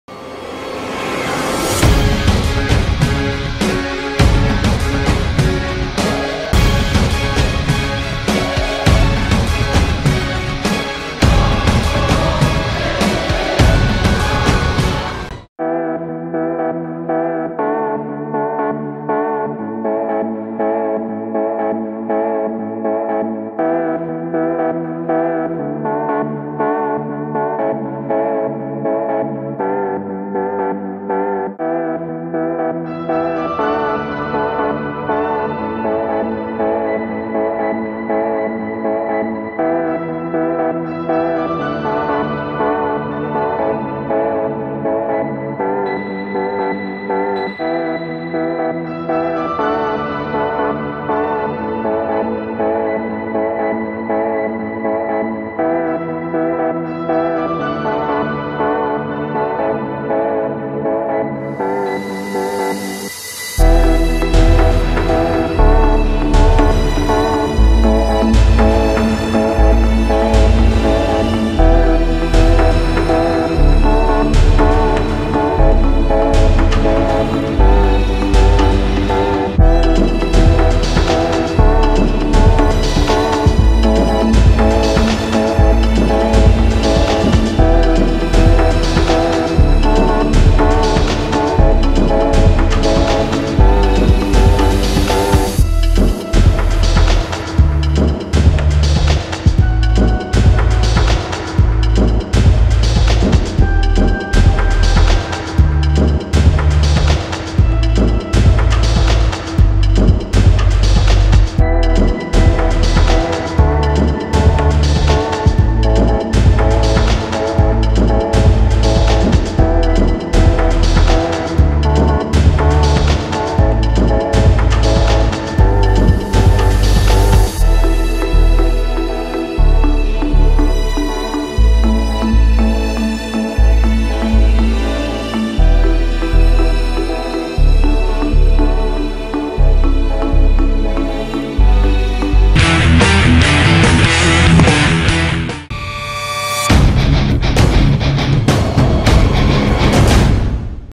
HEAVY Machine Gun Airsoft PKM 6mm AK with BOX Mag, is this the Best AEG for Gunner ?
PKM